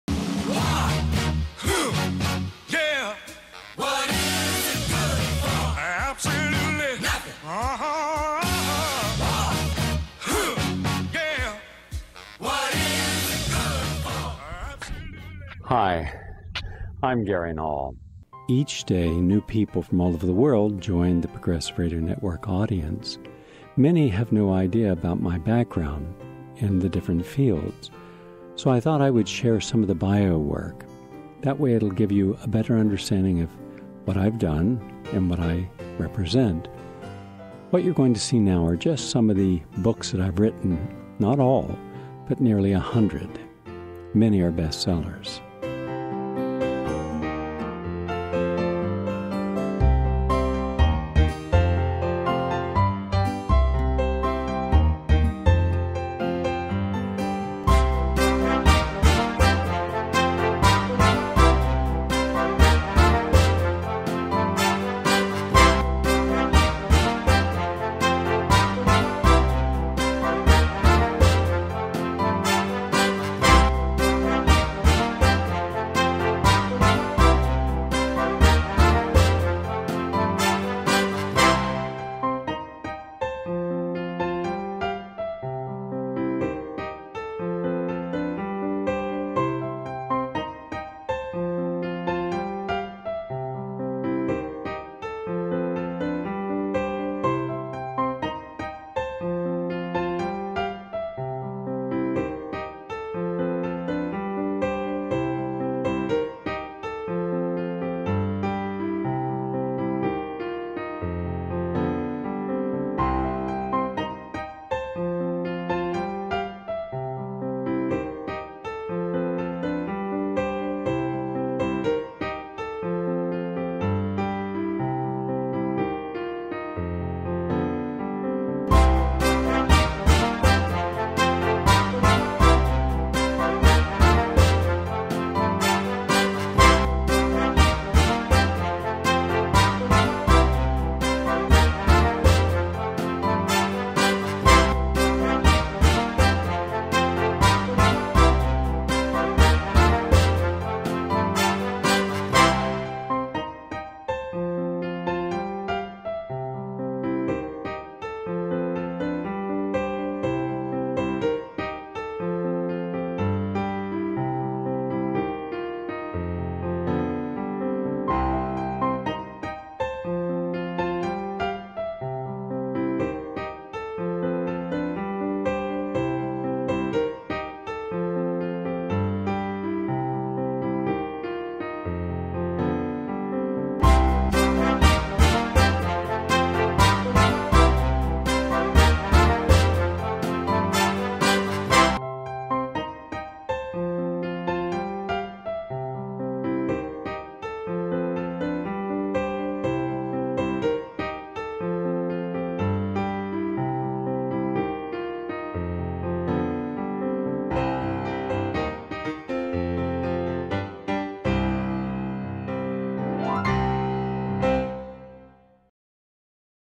Gary Null Author Bio
Learn about the Best-Selling Author, Multi-Award Winning Documentary Filmmaker, and daily Host of the nationally syndicated Gary Null Show.